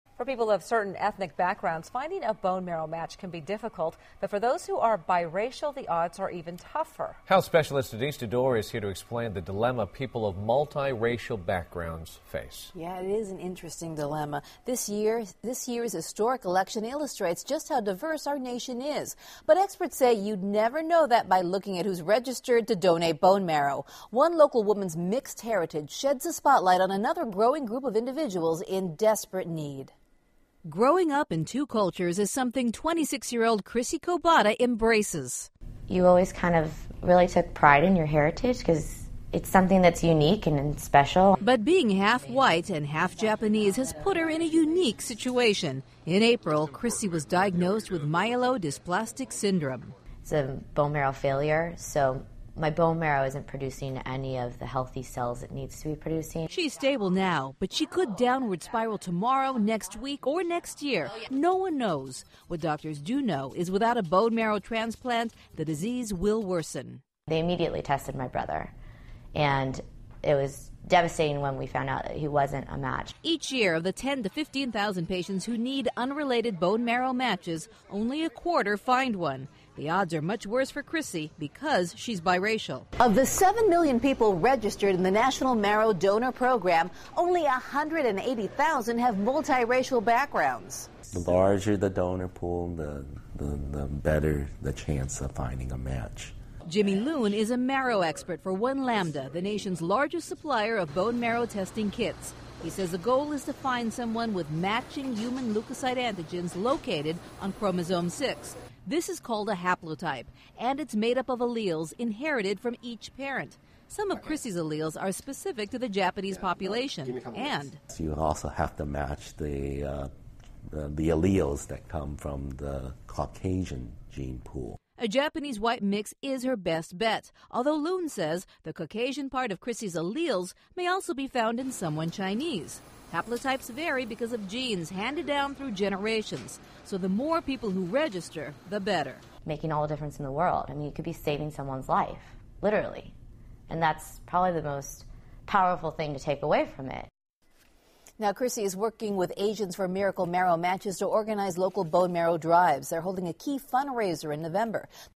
Bi-Racial Bone Marrow Hard To Find
Each year, of the 10,000 to 15,000 patients who need an unrelated bone marrow match only a quarter find one. The odds are much worse for Krissy because she's bi-racial.

Of the seven million people in the National Marrow Donor Program only 180,000 have multi-racial backgrounds.

"The larger the donor pool the better chance for finding a match," said Jimmy Loon.

Jimmy Loon is a marrow expert for One Lambda, the nation's largest supplier of bone marrow testing kits. He says the goal is to find someone with matching human leukocyte antigens located on chromosome six.

This is called a haplo-type and it's made up of alleles inherited from each parent.

Some of Krissy's alleles are specific to the Japanese population.

"You also have to match the alleles that come from the Caucasian gene pool," said Loon.

A Japanese/Caucasian mix is her best bet, although Loon says the Caucasian part of Krissy's alleles may also be found in someone Chinese.

Haplo-types vary because of genes handed down thru generations. So the more people who register, the better.